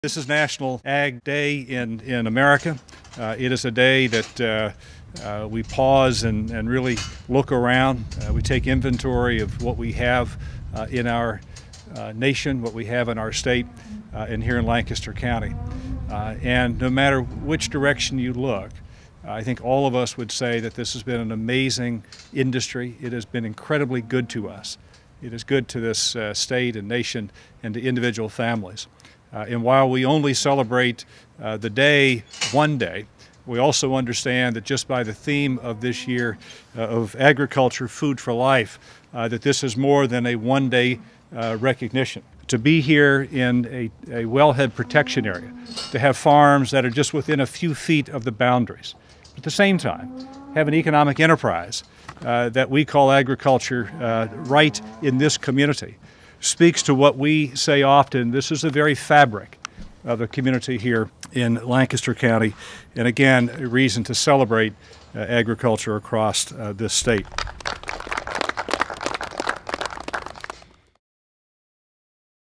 National Agriculture Day
Agriculture Secretary Russell Redding celebrated National Ag Day on Tuesday March 22.  By touring Brubaker Farms in Mt. Joy Lancaster County.